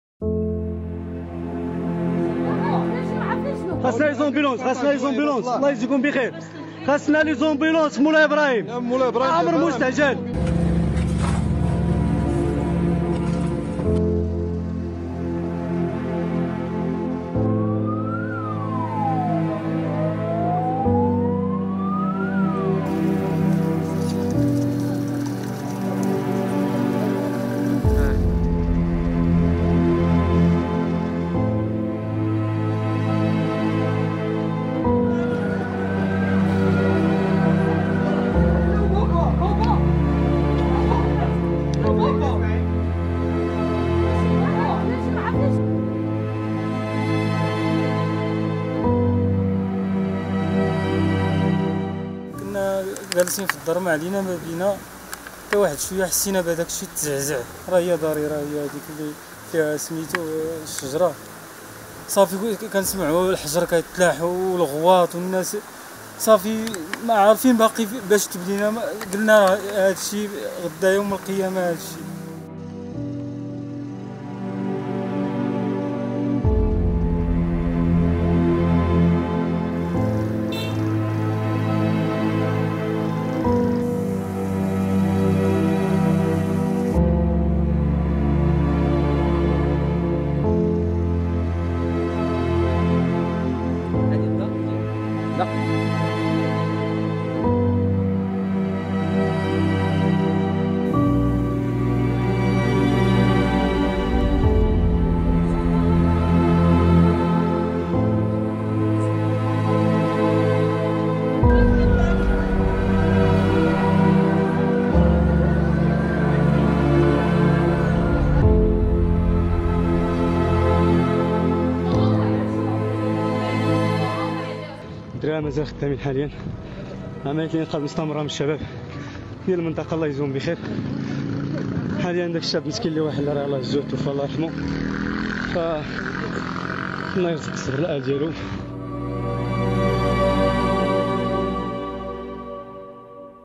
"مولاي إبراهيم" المنكوبة تبكي ضحايا أعنف زلزال في المغرب
كانت مقصداً سياحياً جبلياً.. قرية مولاي إبراهيم المنكوبة تبكي ضحايا أعنف زلزال في
لمعرفة أحدث المستجدات.. عبر البث المباشر من هنا ⤵
وبإمكانكم متابعة العربية على منصات التواصل